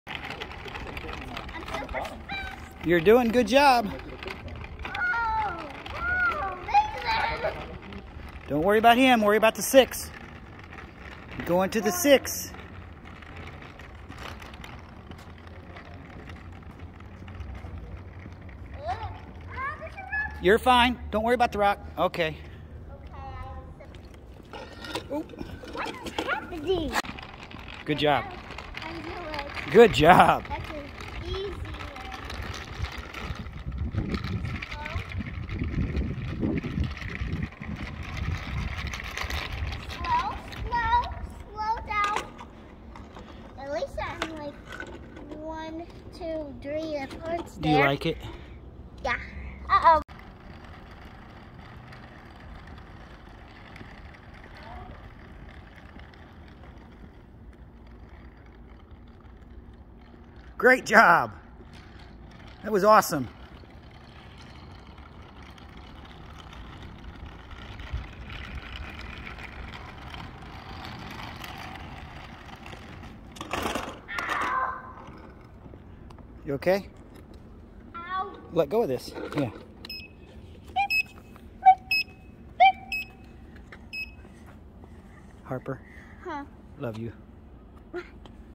EUC Training - Hour 2
Training my six-year-old granddaughter to ride an EUC. Hour #2